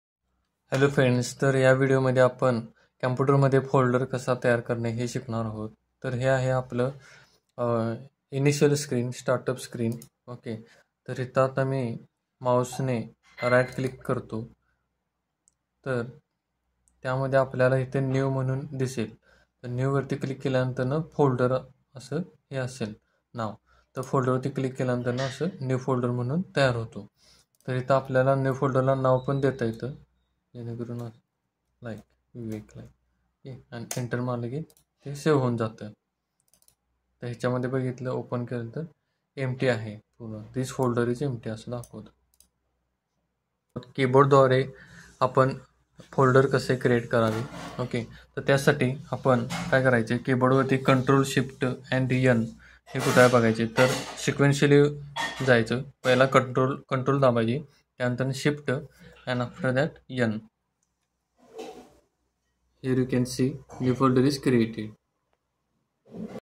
Create new folder using both technics: using keyboard and mouse
In this video, you can learn how to create a new folder using keyboard and mouse.